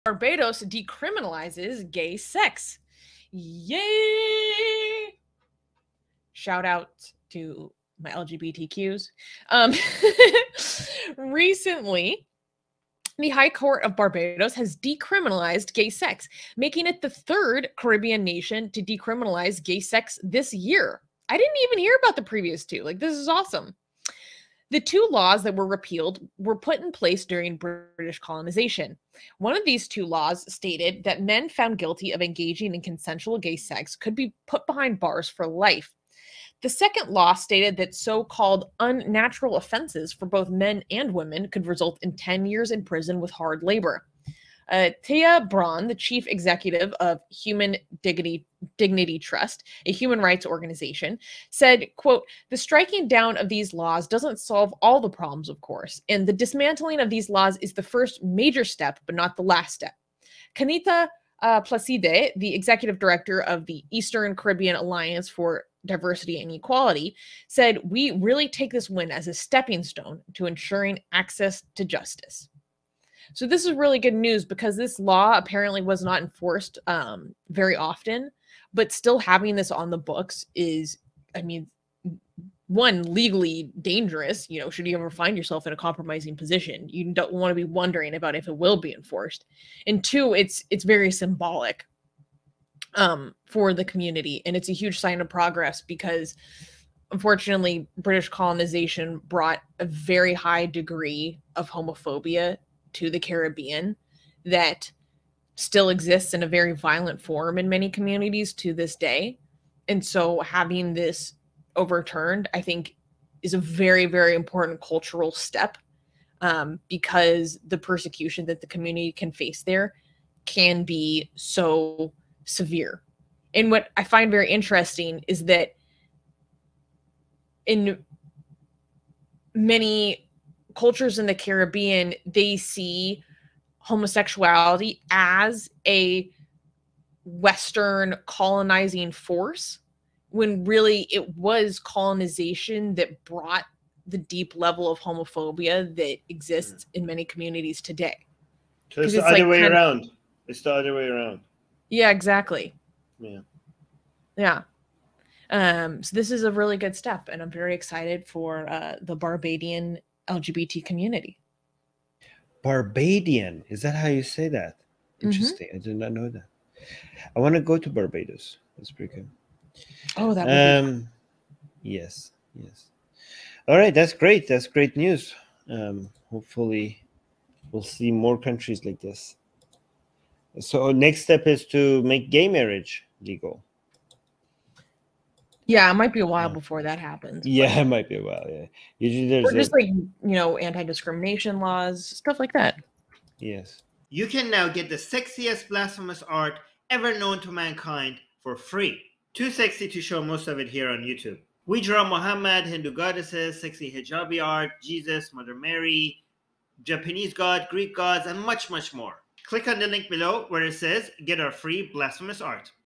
Barbados Decriminalizes Gay Sex!
Recently, The High Court of Barbados has decriminalized gay sex making it the third Caribbean nation to decriminalize gay sex this year! The two laws that were repealed were put in place during the British colonization. One of these laws stated men found guilty of engaging in consensual gay sex could be put behind bars for life. The second law stated that so-called “unnatural offenses” for both men and women could result in ten years in prison with hard labor. Téa Braun, the Chief Executive of Human Dignity Trust, a human rights organization, said, "The striking down of these laws doesn't solve all problems, of course," and "The dismantling of these laws is the first major step, but not the last step." Kenita Placide, the Executive Director of the Eastern Caribbean Alliance for Diversity and Equality, Inc (ECADE), said, "We really take this win as a stepping stone to ensuring access to justice."

Source:
Location: Barbados